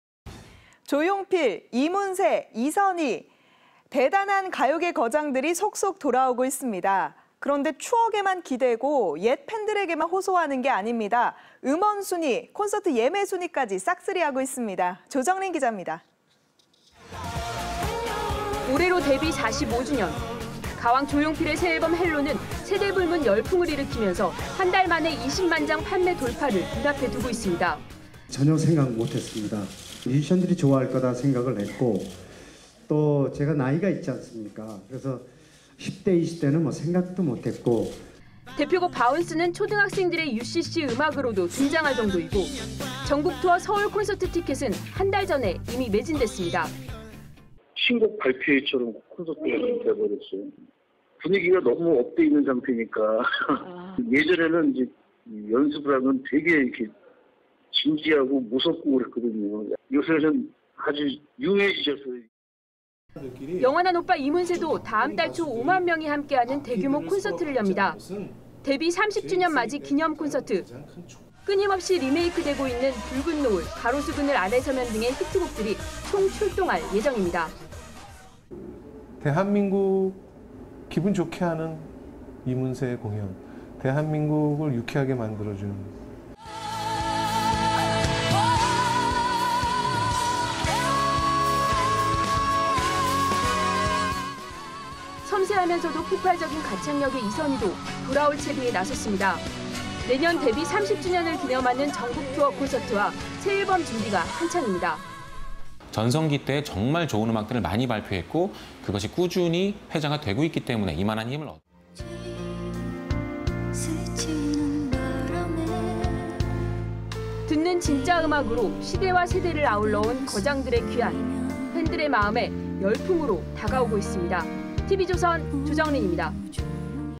조용필부터 이문세·이선희까지...거장들의 귀환
조용필, 이문세, 이선희, 이름만 들어도 존재감이 대단한 가요계 거장들이 속속 돌아오고 있습니다. 그런데 추억에만, 옛 팬들에게만 호소하는 게 아닙니다. 음원 순위, 콘서트 예매 순위까지 싹쓸이하고 있습니다.